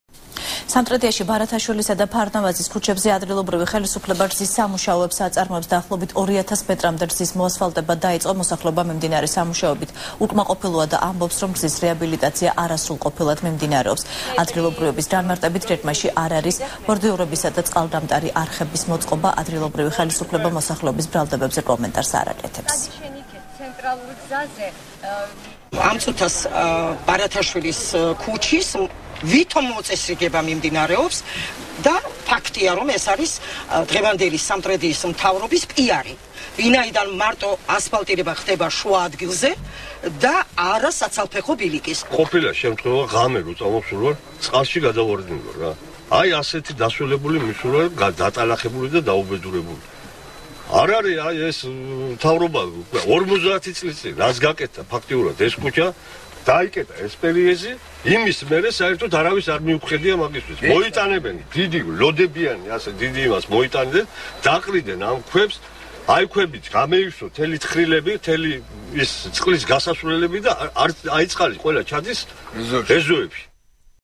ახალი 12 | უკმაყოფილო მოსახლეობა | 08.08.12
სამტრედიაში ბარათაშვილის და ფარნავაზის ქუჩებზე ადგილობრივი ხელისუფლება გზის სამუშაოებს  აწარმოებს. დაახლოებით 2000 მეტრამდე გზის მოასფალტება  დაიწყო. მოსახლეობა მიმდინარე სამუშაოებით უკმაყოფილოა და ამბობს, რომ გზის რეაბილიტაცია არასრულყოფილად  მიმდინარეობს. ადგილობრივების განმარტებით,  გეგმაში არ არის ბორდიურებისა  და წყალგამტარი არხების მოწყობა. ადგილობრივი ხელისუფლება მოსახლეობის ბრალდებებზე კომენტარს არ აკეთებს.